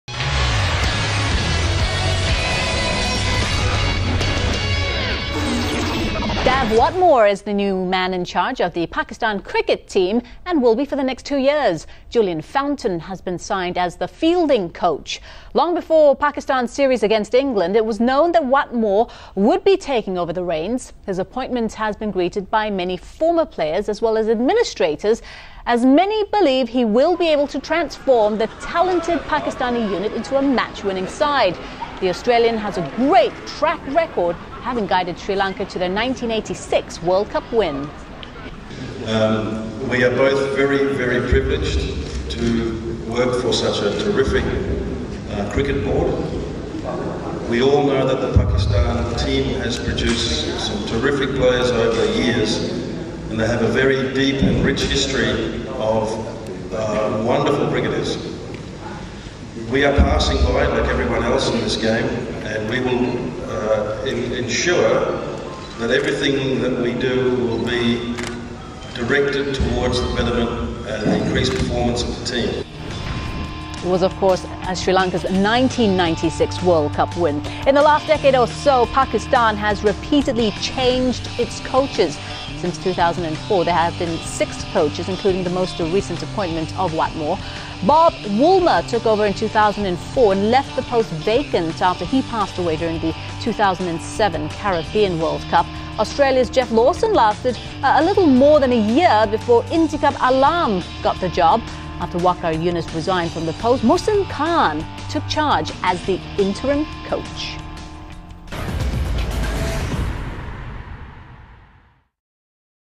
Dav Whatmore appointed as Pakistan coach
The Pakistan Cricket Board appointed Australian Dav Whatmore as the new coach of the national team on a two-year contract. His first test will be the four-nation Asia Cup, starting March 11 in Bangladesh that also includes defending champion India and Sri Lanka.